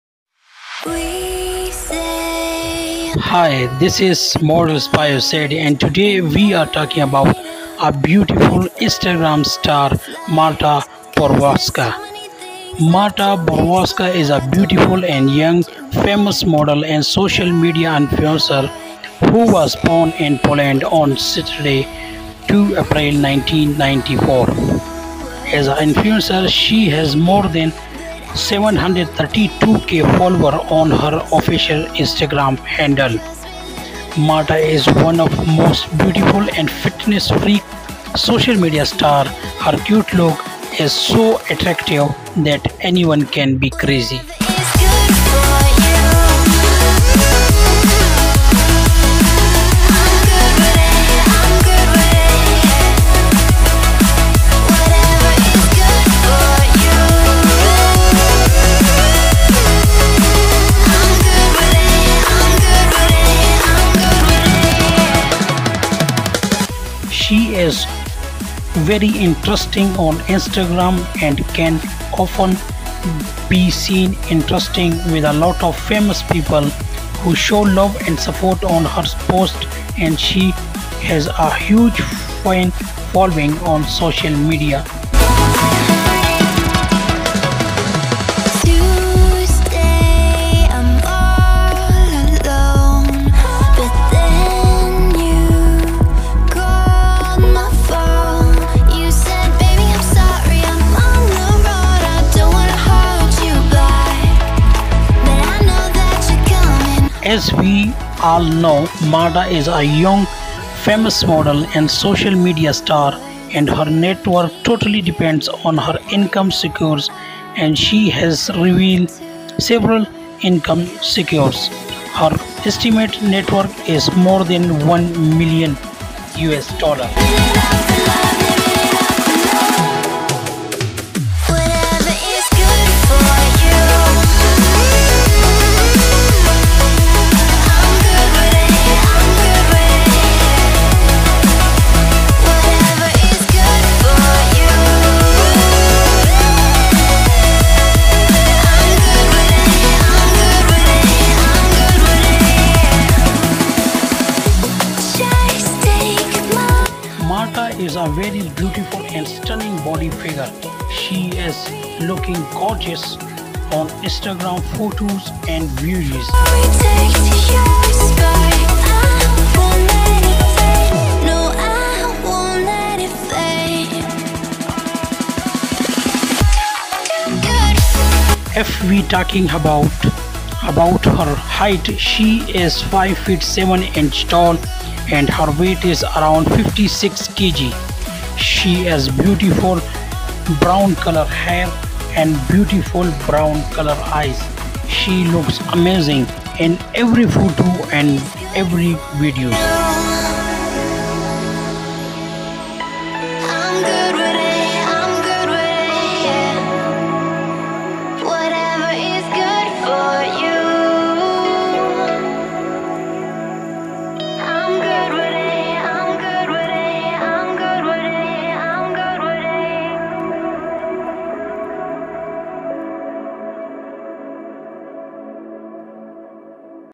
Marta Borkowska Polish model Biography | Instagram star, wiki , TikTok star
In this video, we're going to take a look at Marta Borkowska, an Polish model , Instagram star and social media celebrity. We'll learn about her biography, wiki, age, lifestyle, net worth and more.

If you're interested in seeing more of Marta Borkowska work, or want to know more about this model and Instagram, then be sure to check out this video! We'll provide you with all the information you need to get to know Marta Borkowska better.

All of the information was taken from our website & google search.